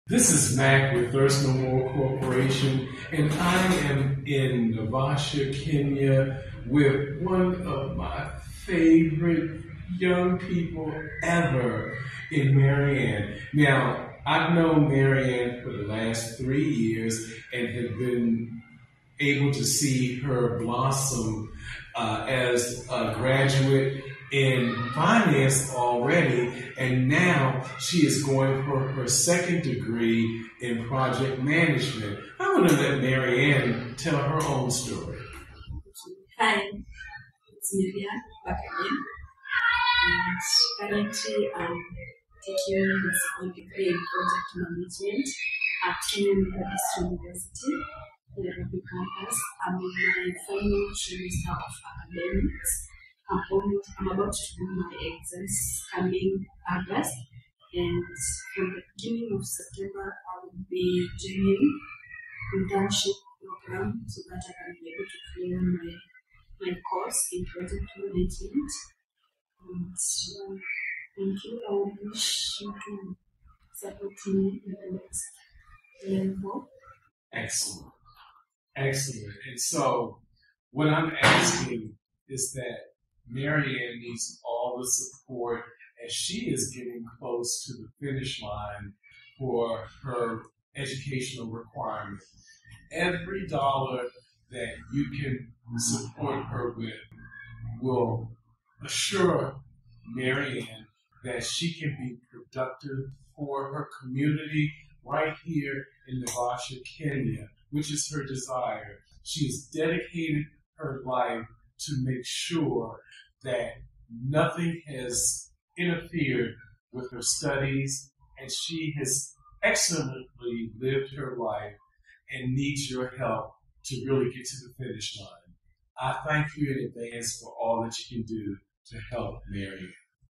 MARYANN WANGUI EDUCATION UPDATE AUGUST 2024
Here is an update on our Maryann as she completes her current semester studies in Project Management. She previously has a BA in Finance. All of her studies have been at @kenyamethodistuniversity5918